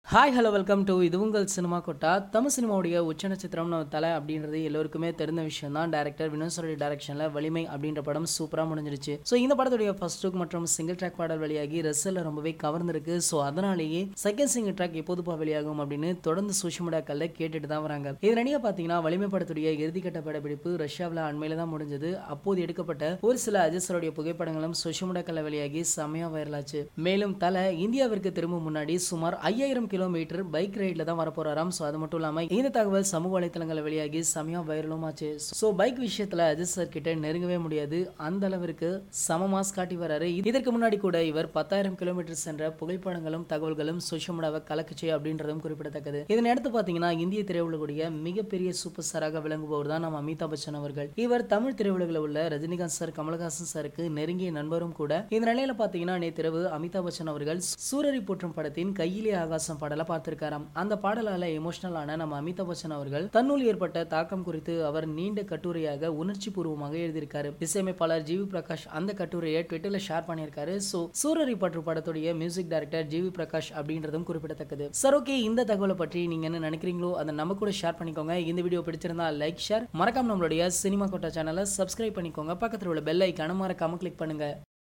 "அஜித் கிட்ட இதுல நெருங்கவே முடியாது" உணார்ச்சிபூர்வமாக பேசிய அமிதாப் பச்சன்| Ajith |Amitha Bachan
"அஜித் கிட்ட இதுல நெருங்கவே முடியாது" உணார்ச்சிபூர்வமாக பேசிய அமிதாப் பச்சன்| Ajith Kumar |Amitha Bachan |H Vinoth | Boney Kapoor